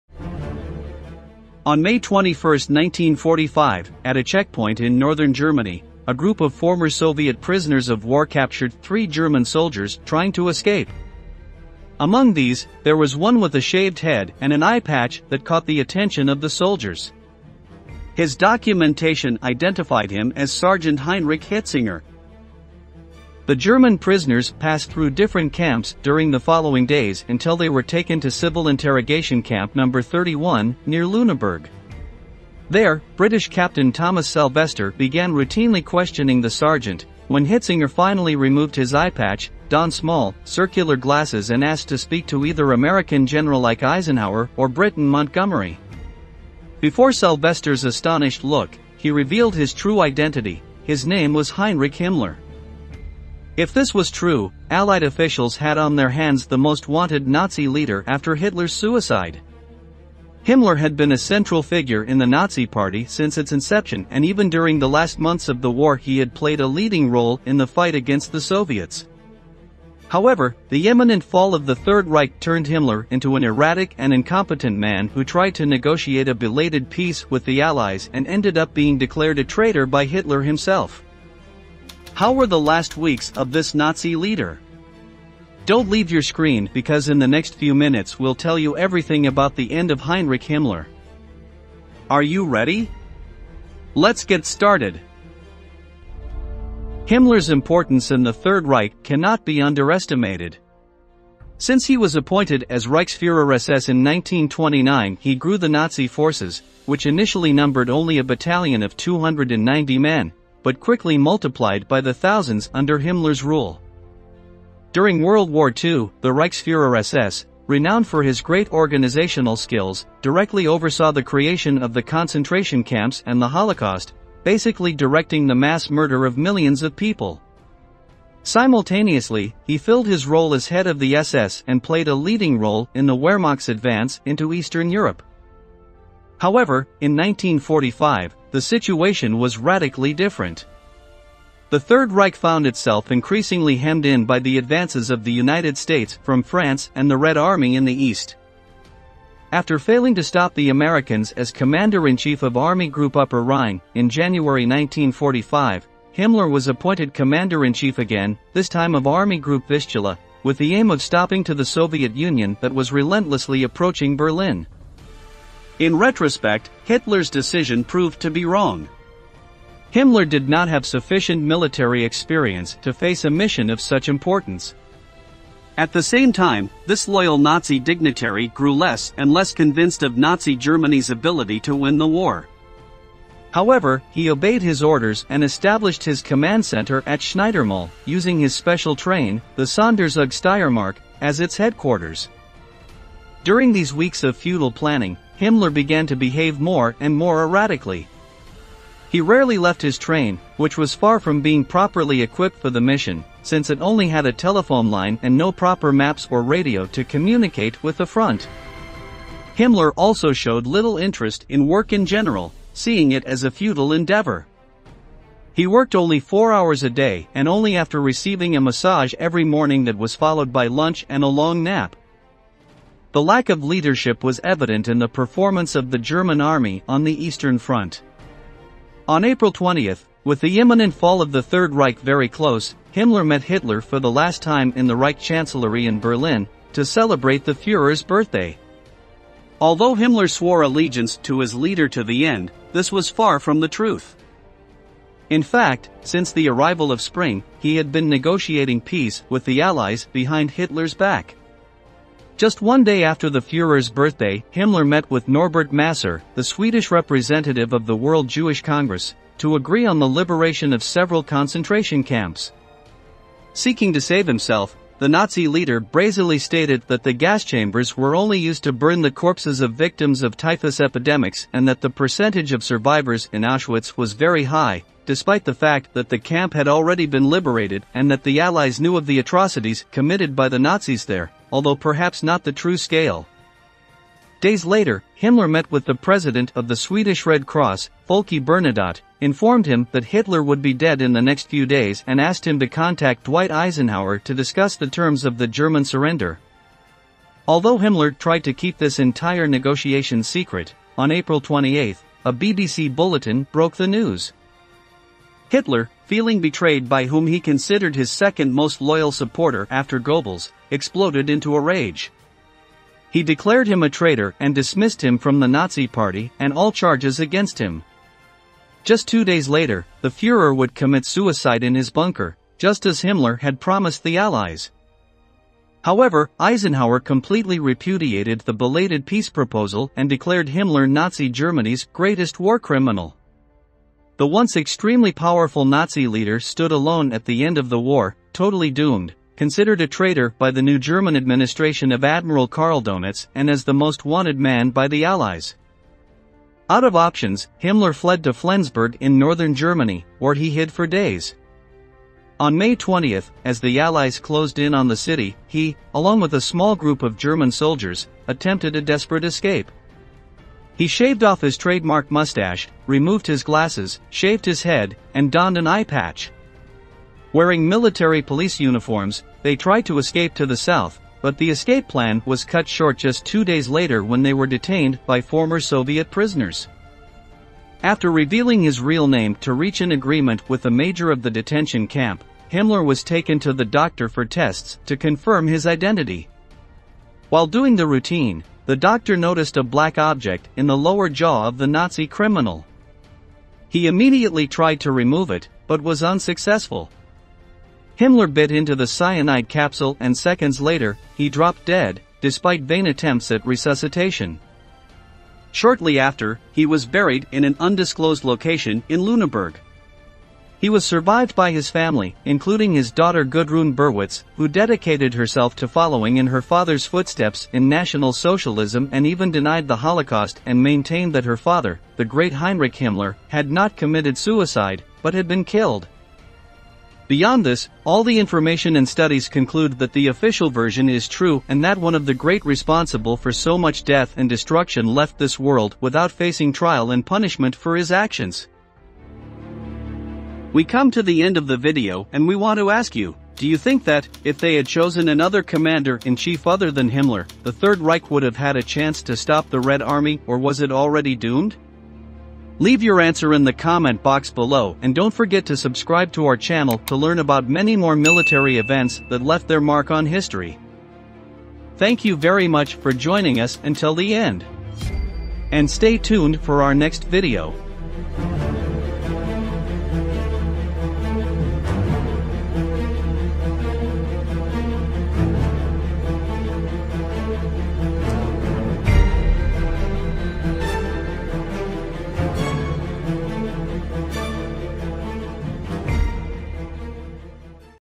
Himmler: What were the LAST days of the SS leader's LIFE like?
How were the final days of one of the top Nazi leaders? If you want to discover all the details about the end of Heinrich Himmler, don't miss this video!
On May 21, 1945, at a checkpoint in northern Germany, a group of former Soviet prisoners of war captured three German soldiers who tried to escape. Among these, there was one with a shaved head and an eye patch that caught the attention of the soldiers. His documentation identified him as Sergeant Heinrich Hitzinger.

🔴 WATCH THE VIDEO OF THE CHANNEL: "MILITARY MIGHT"